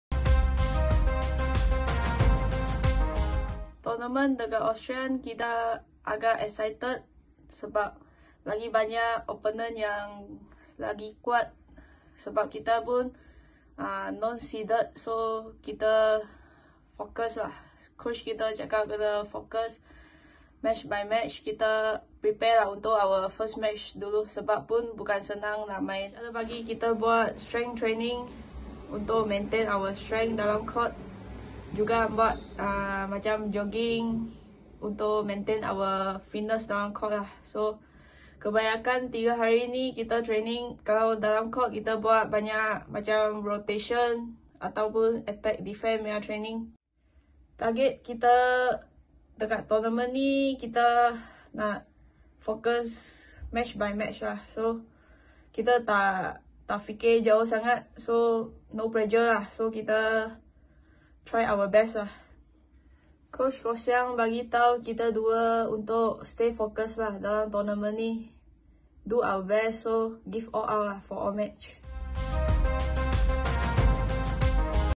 Badminton | Austria Open | Toh Ee Wei
Selepas merangkul kejuaraan di kejohanan Antarabangsa Slovenia, Toh Ee Wei melihat saingan Terbuka Austria 2021 yang akan bermula esok lebih sukar kerana barisan pemain pemain yang mengambil bahagian lebih sukar.